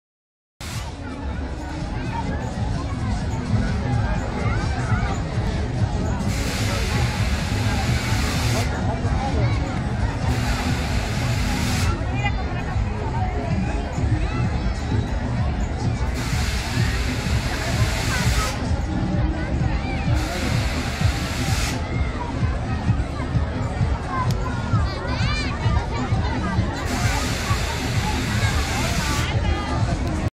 2 de las Atracciones infantiles en las Fiestas de La Melonera 2013 en Madrid.
Ferias y fiestas de La Melonera ( Madrid ). fiestas del 12 al 22 de Septiembre 2013, recinto ferial junto al Matadero, en el Paseo de la Chopera de Madrid, metro Legazpi. Conciertos gratuitos de Melocos y Bertín Osborne. días del niño con atracciones al 50%, martes 17, miércoles 18 y jueves 19 de Septiembre 2013.